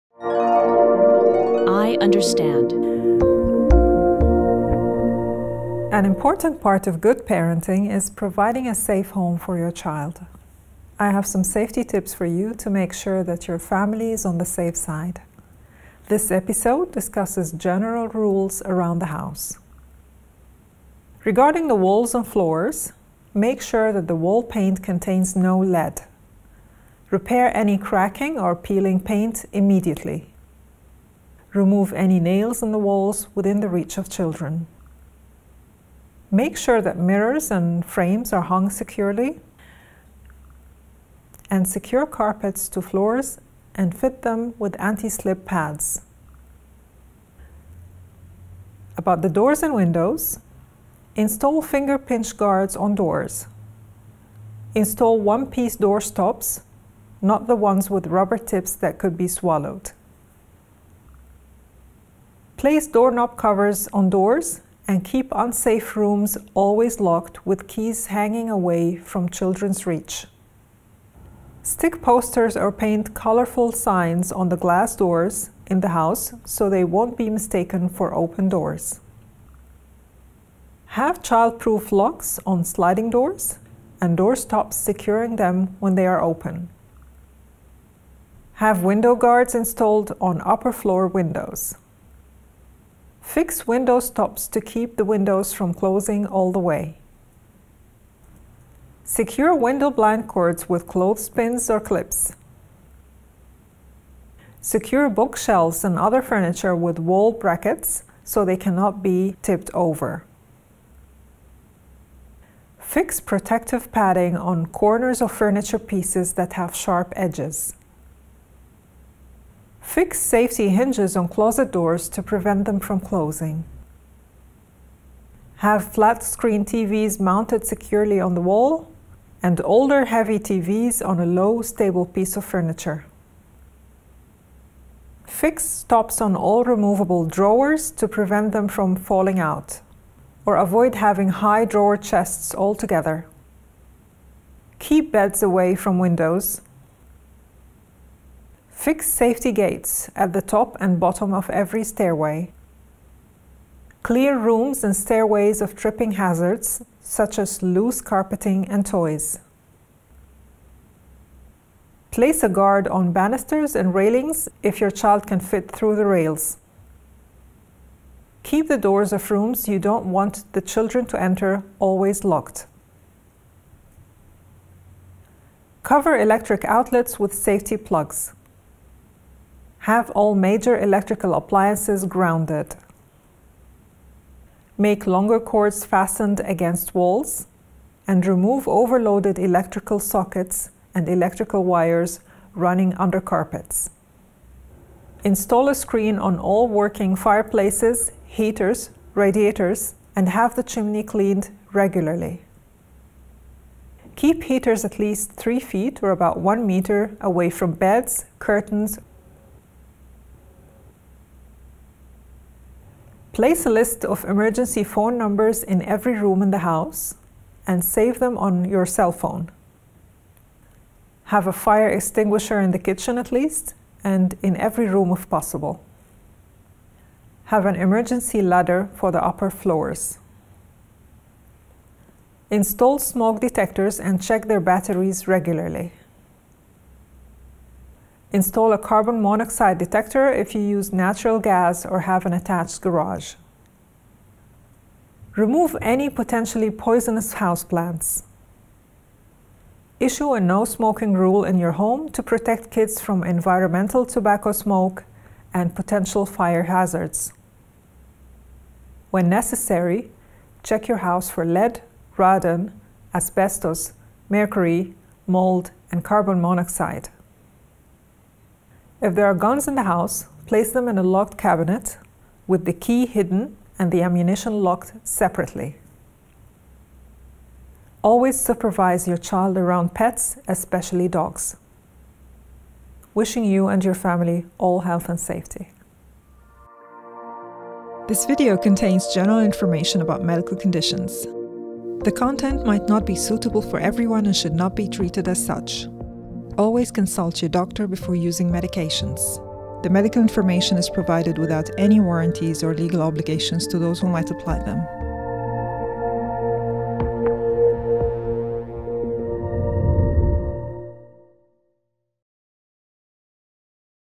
Wall & Floor Safety
An important part of good parenting is providing a safe home for your child.

I have some safety tips for you to make sure that your family is on the safe side.

This episode discusses general rules around the house

Walls & Floors:

- Make sure that the wall paint contains no lead. Repair any cracking or peeling paint immediately.
- Remove any nails in the walls within the reach of children.
- Make sure that mirrors and frames are hung securely.
- Secure carpets to floors and fit them with anti-slip pads.

Doors & Windows:

- Install finger-pinch guards on doors.
- Install one-piece door stops (not the ones with rubber tips that could be swallowed)
- Place doorknob covers on doors, and keep unsafe rooms always locked with keys hanging away from children’s reach.
- Stick or paint colorful sings on all glass doors in the house so they won't be mistaken for open doors.
- Have childproof locks on sliding doors, and door stops securing them when they are open.
- Have window guards installed on upper-floor windows.
- Fix window stops to keep the windows from closing all the way.
- Secure window-blind cords with clothespins or clips.

Furniture:

- Secure bookshelves and other furniture with wall brackets so they can't be tipped over.
- Fix protective padding on corners of furniture pieces that have sharp edges.
- Fix safety hinges on closet doors to prevent them from closing.
- Have flat screen TVs mounted securely on the wall, and older, heavy TVs on a low, stable piece of furniture.
- Fix stops on all removable drawers to prevent them from falling out. Or avoid buying drawer chests all together.
- Keep beds away from windows.

Stairways:

- Fix safety gates at the top and bottom of every stairway.
- Clear stairways of tripping hazards, such as loose carpeting and toys.
- Placed a guard on banisters and railings if your child can fit through the rails.
- Keep the doors of rooms you don’t want the children to enter always locked.

Electrical:

- Cover electric outlets with safety plugs.
- Have all major electrical appliances grounded.
- Make longer cords fastened against walls.
- Remove overloaded electrical sockets and electrical wires running under carpets.
- Have televisions screwed to the wall or placed out of children’s reach on sturdy TV tables.

Heating & Cooling Elements:

- Install a screen on all working fireplaces/heaters/radiators.
- Have the chimney cleaned regularly.
- Keep heaters at least 3 feet (about 1 meter) from beds, curtains, or anything flammable.

Emergency Equipment & Numbers:

- Place a list of emergency phone numbers in every room in the house, and save them on your cell phone.
- Have a fire extinguisher in the kitchen.
- Have an emergency ladder for the upper floors.
- Have smoke detectors and check their batteries regularly.
- Install a carbon monoxide detector if you use natural gas or have an attached garage.

Other Safety Issues:

- Remove any potentially poisonous houseplants.
- Issue a no-smoking rule in your home to protect kids from environmental tobacco smoke and potential fire hazards.
- When necessary, check your house for lead, radon, asbestos, mercury, mold, and carbon monoxide.
- If there are guns in the home, place them in a locked cabinet with the key hidden and the ammunition locked separately.
- Always supervise your child around pets, especially dogs.

Wishing you and your family all health and safety.